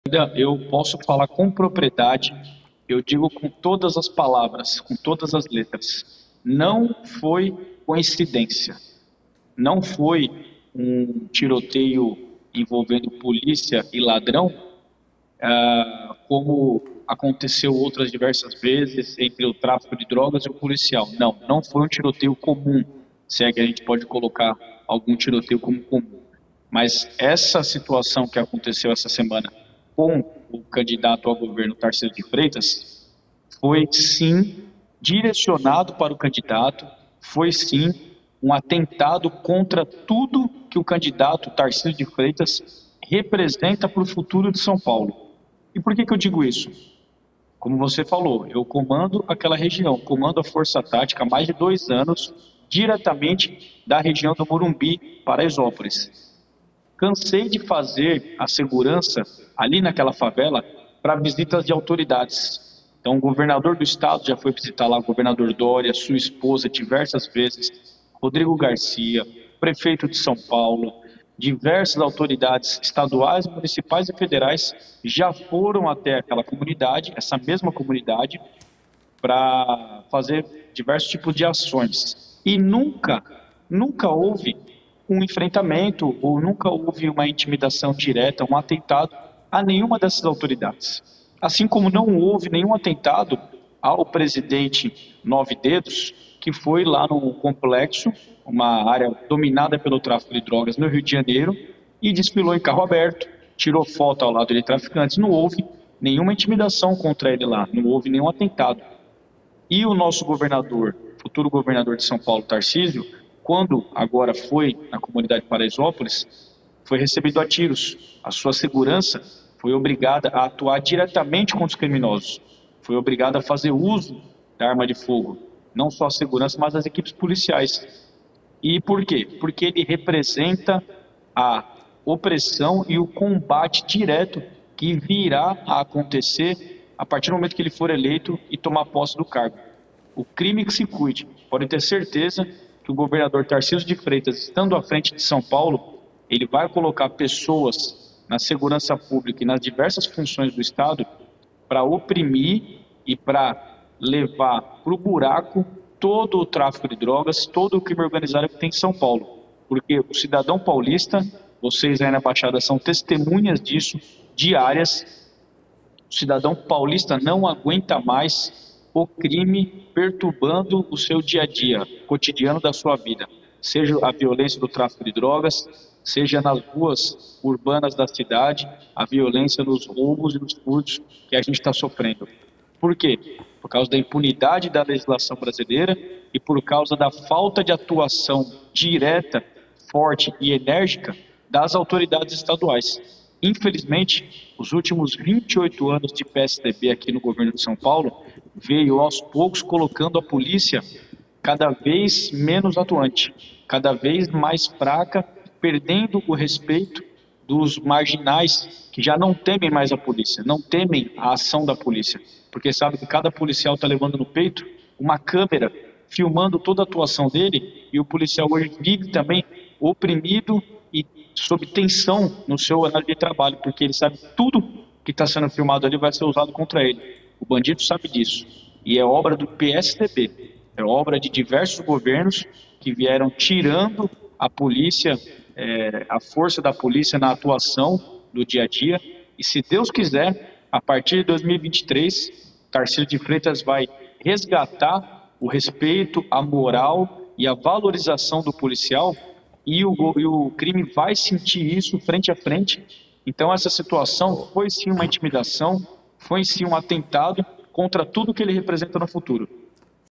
"Foi um atentado contra tudo que o Tarcísio representa para o futuro de SP", disse Capitão Telhada
Nossos Canais:
Tv Cultura Litoral: